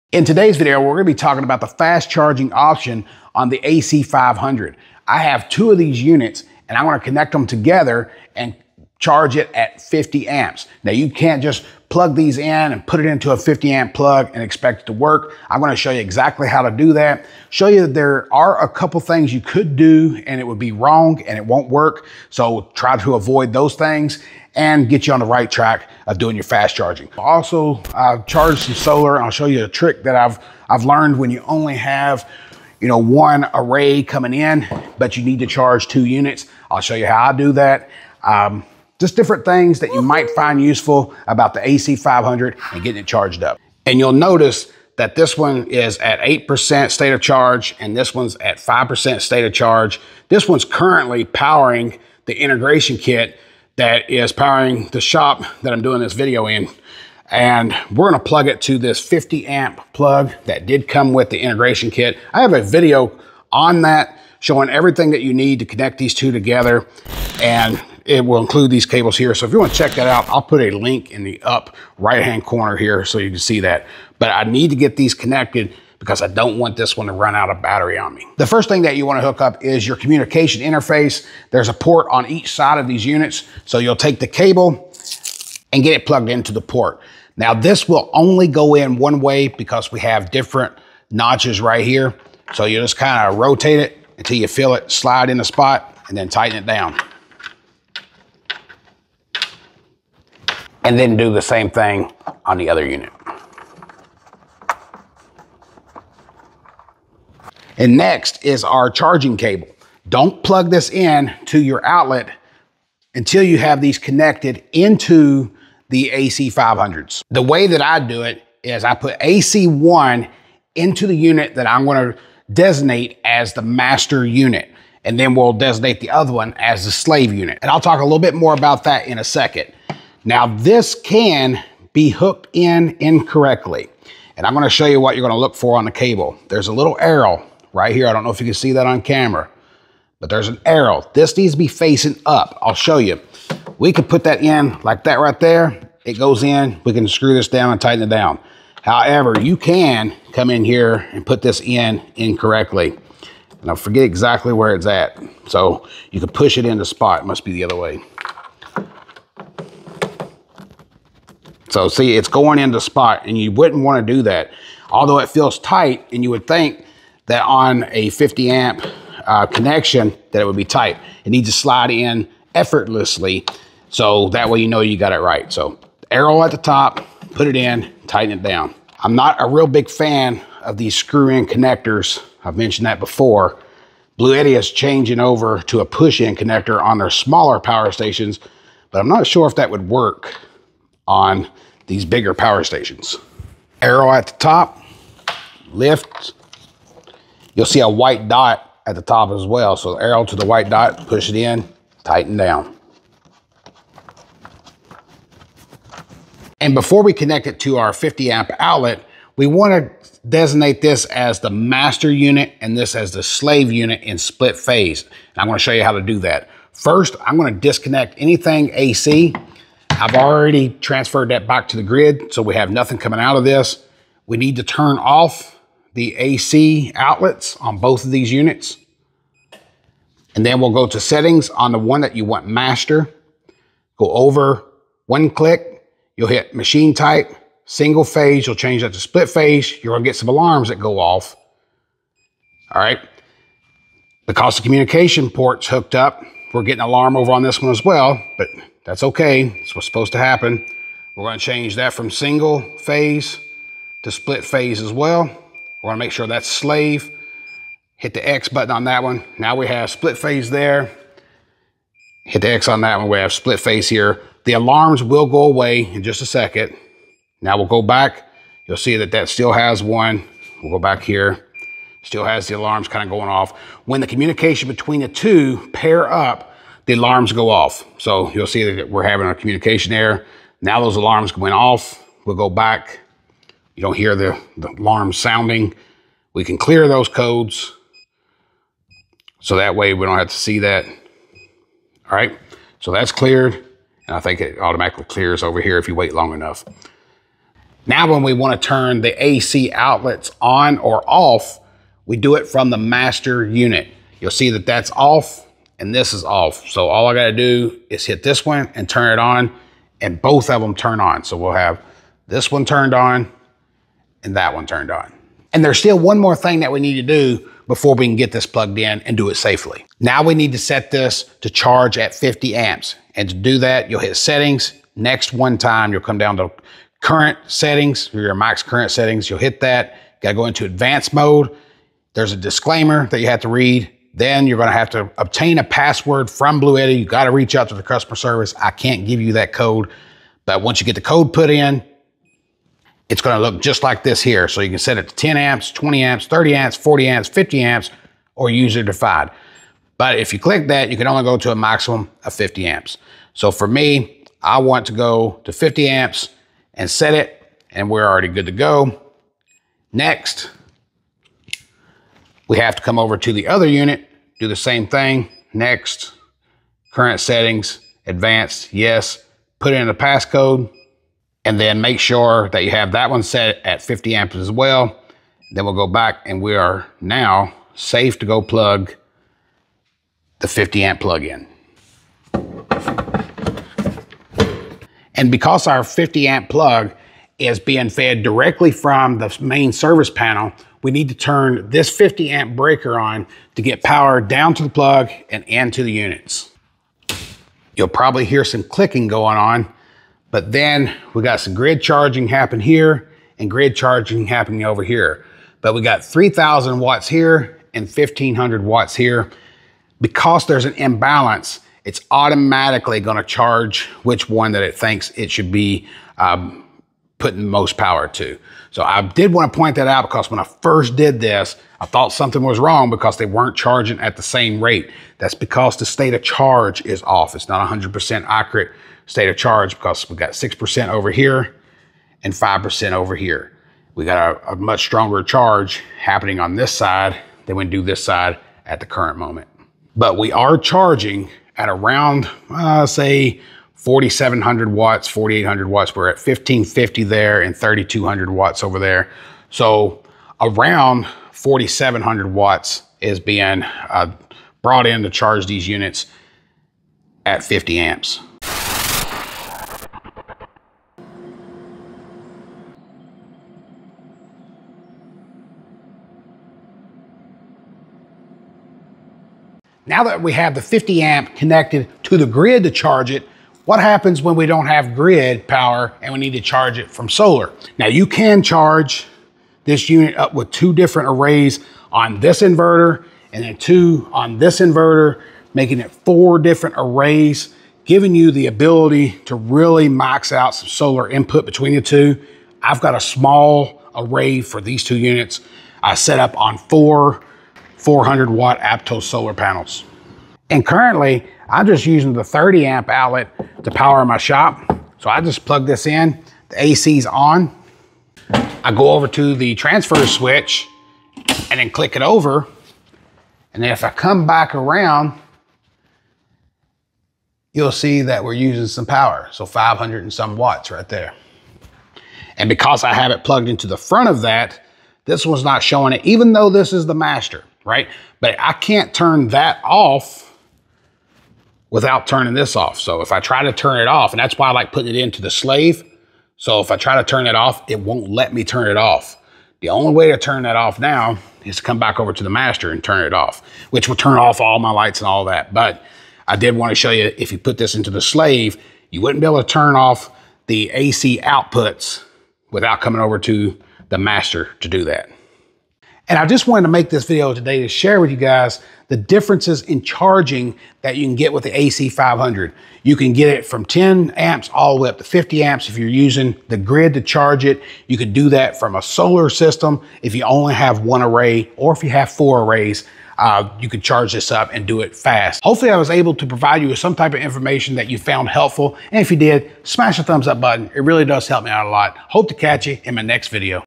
SUPERCHARGE Your Bluetti AC500: Unlock LIGHTNING-FAST 50amp Charging!
This is your guide to 50A Fast charging the Bluetti AC500. There are a couple of important things to know before you connect your AC500 power station to a 50-amp outlet. Also, I talk about how I charge both units with just one small solar array.

Popular Power Stations from Bluetti:
Elite 200 V2
AC300 B300K
AC500 B300K

Wearables:
Handsfree 1
Handsfree 2

Not only are you receiving the best prices but you are also working with a team that is customer-focused!